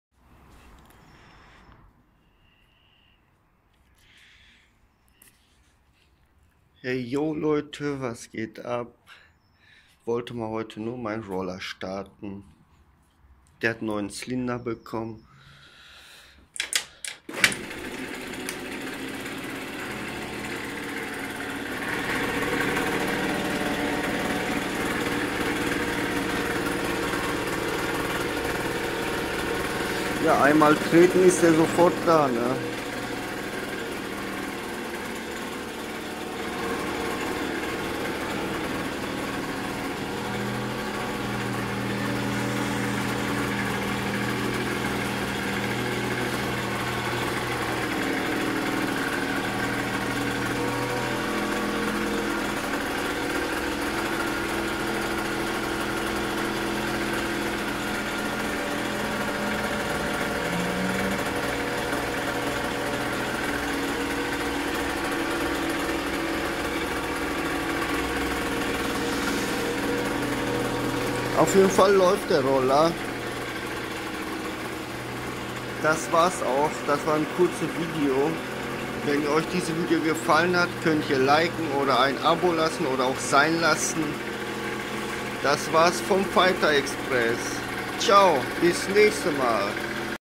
Agm Fighter 50 one Starten mit dem neuen Zylinder
Roller ist vom Baujahr 2011 zylinder wa  kaputt neue wurde eingebaut ich bin sehr zufrieden mit dem Teil